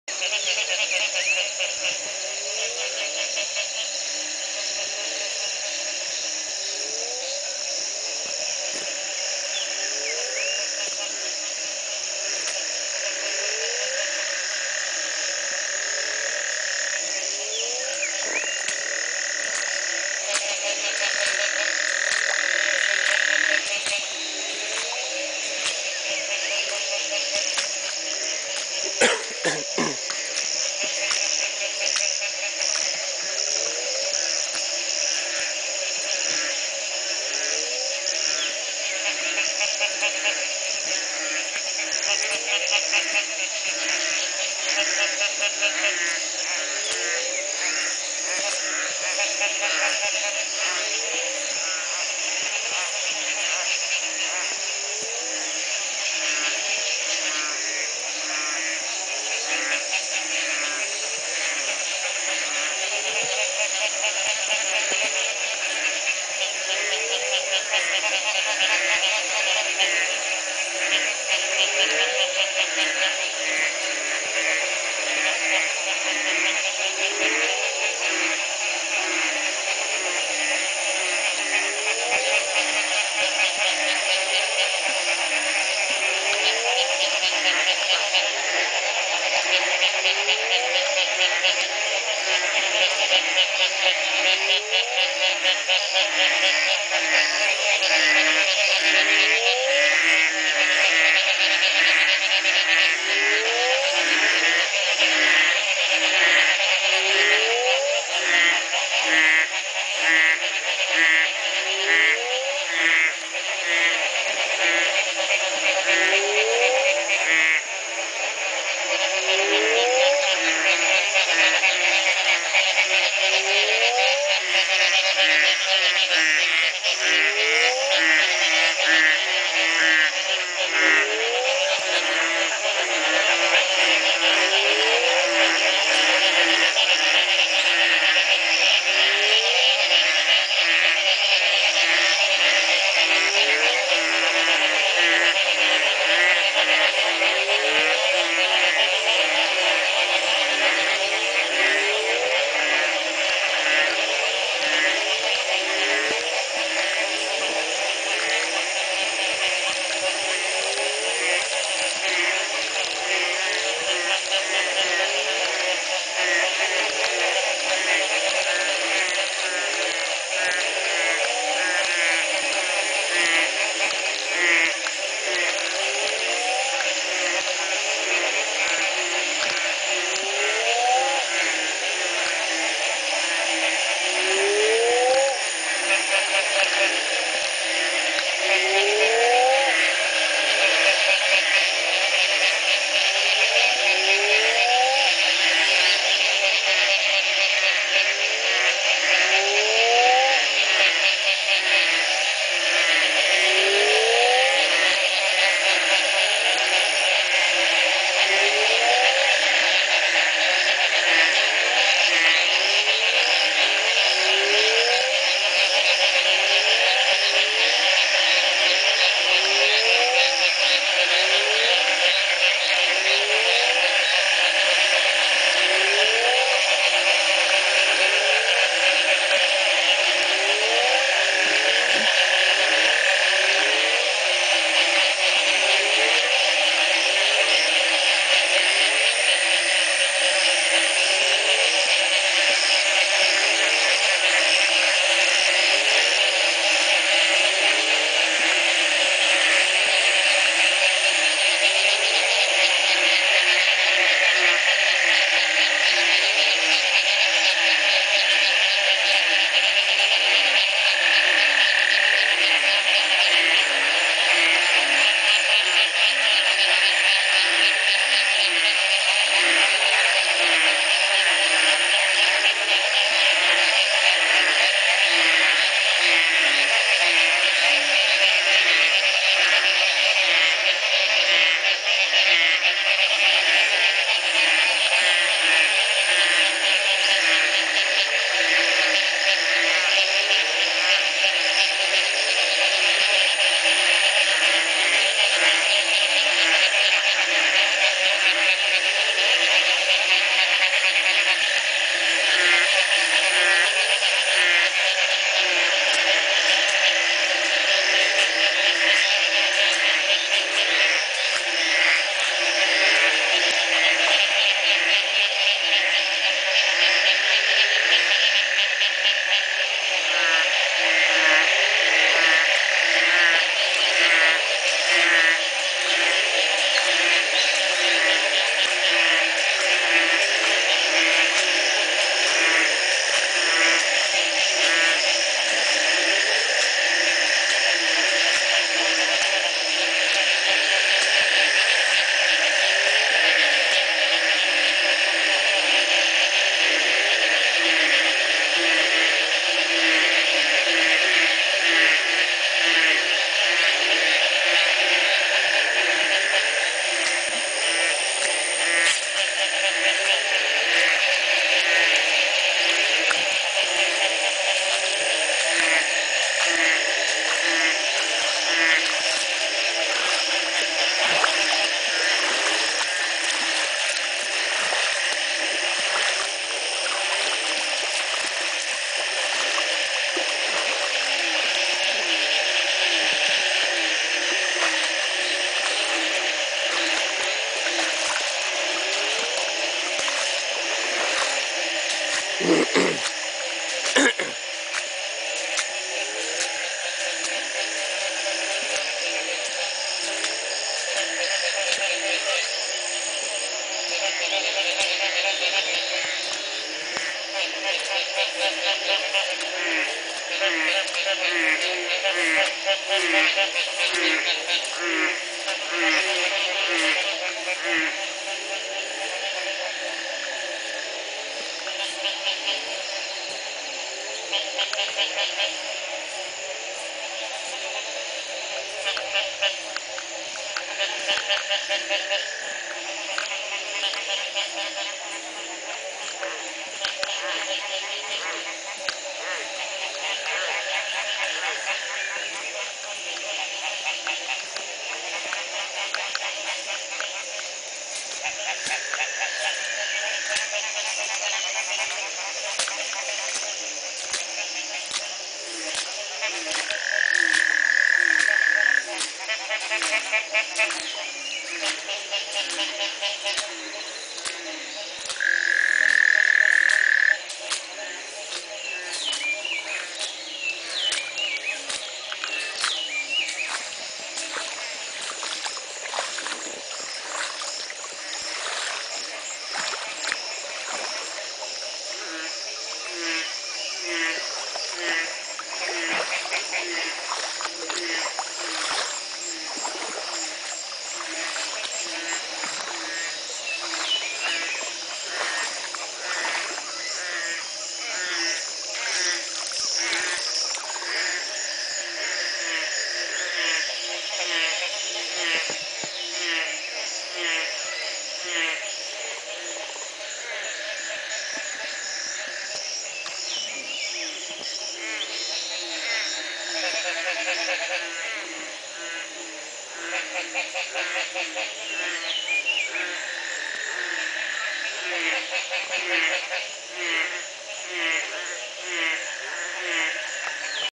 The Community Baboon Sanctuary in Belize. Frog calls galore.MPG
This is a video taken in 2010 from my home in Belize the next morning after a single night of rain fall.Realize that the day before everything was bone dry and huge cracks in the ground from a 3 month drought.Check out more photos